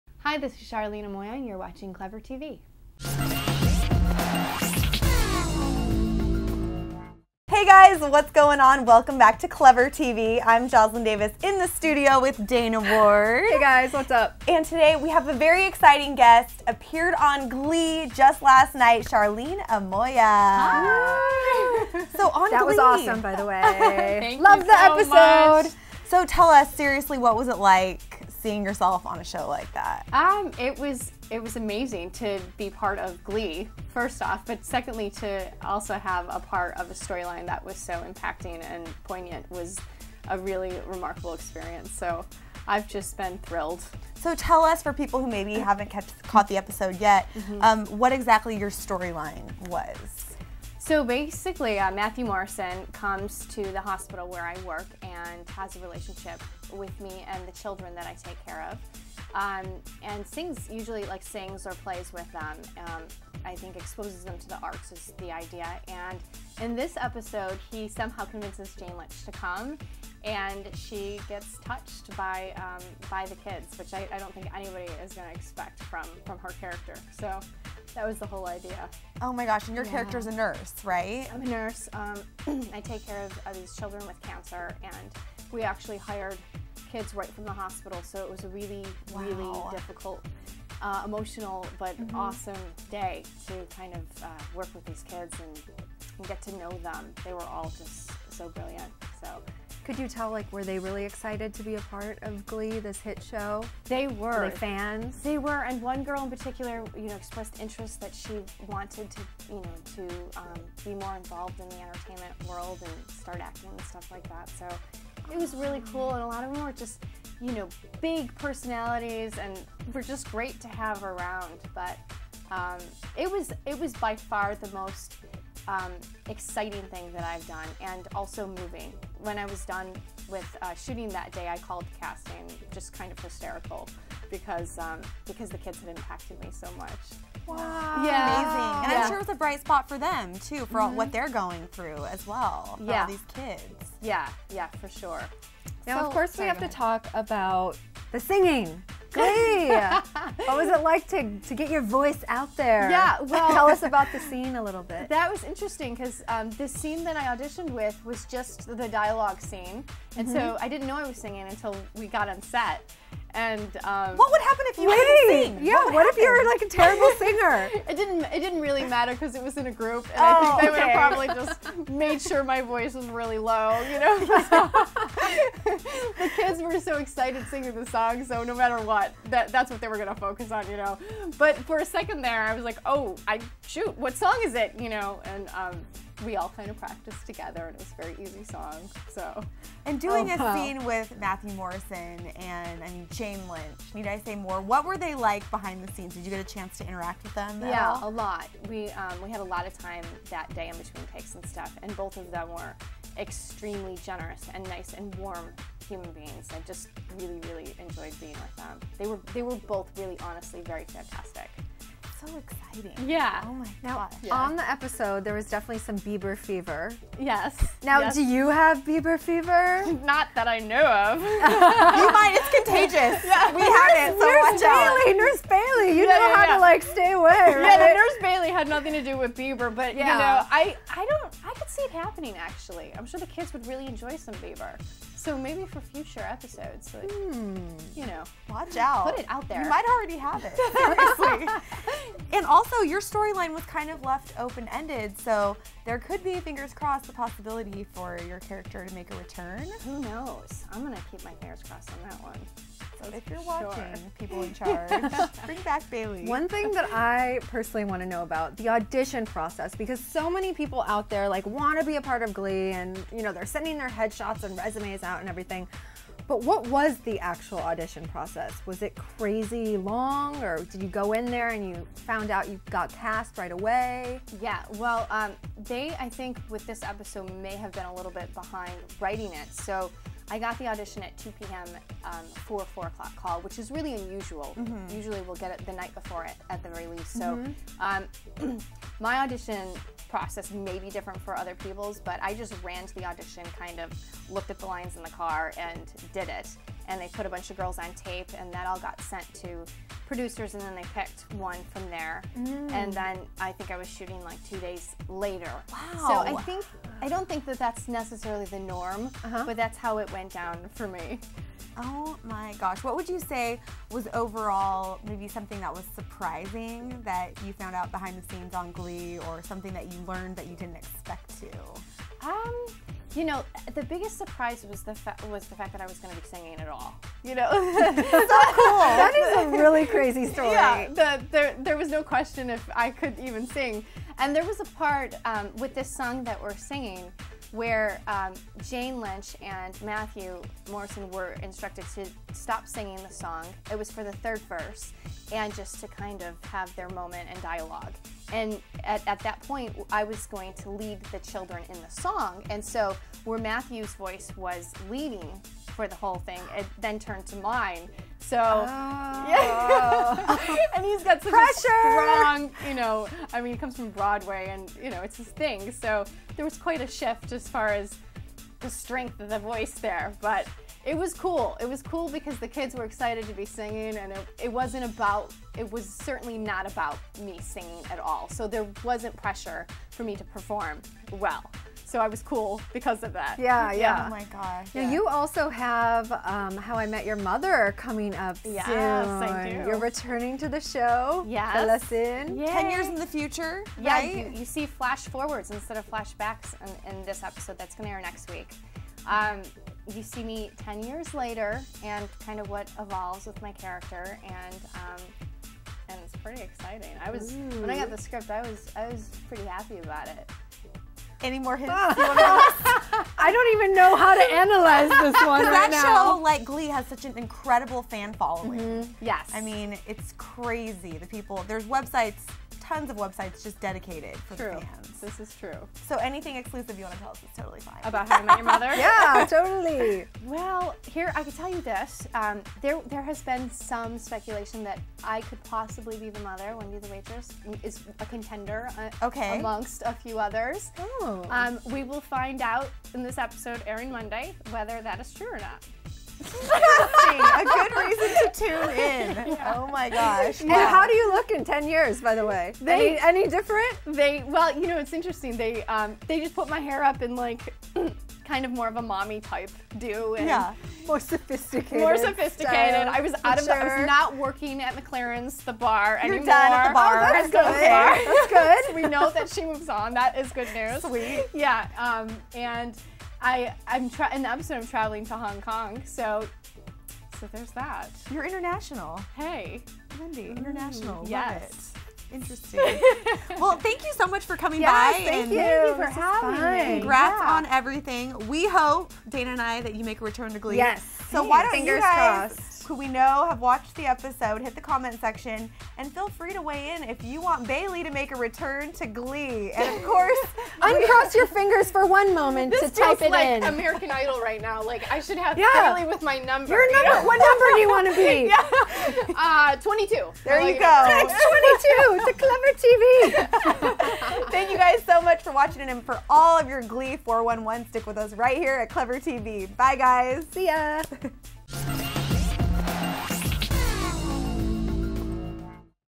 Charlene Amoia: Glee Interview
Charlene Amoia stopped by the ClevverTV Lounge to talk about her guest role on 'Glee', singing on the show, the audition process and the chance of returning to the show.